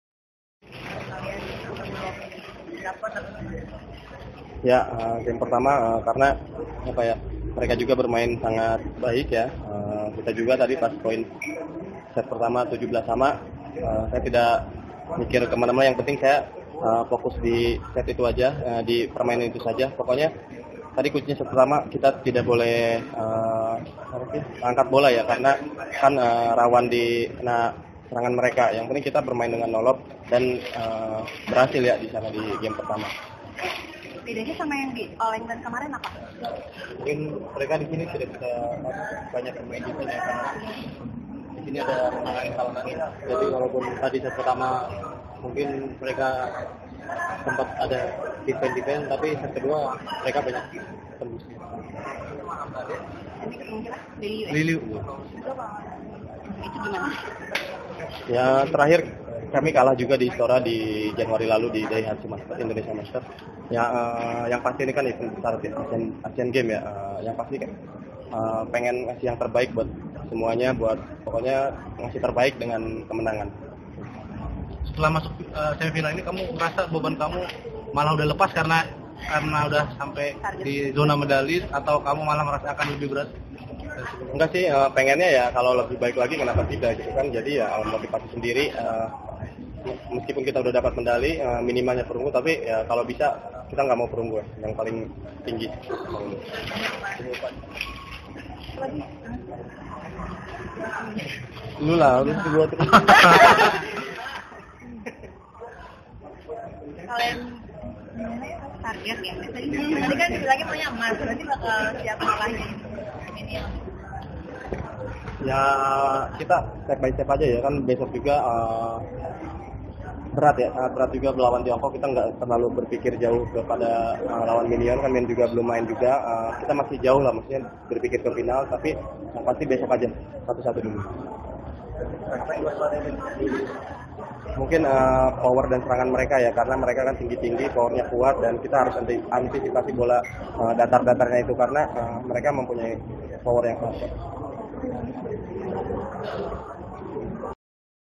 Fajar/Rian ke Semifinal Asian Games, ini komentarny (hanya suara)
Asian Games, 26 Agustus 2018 (Foto : PBSI)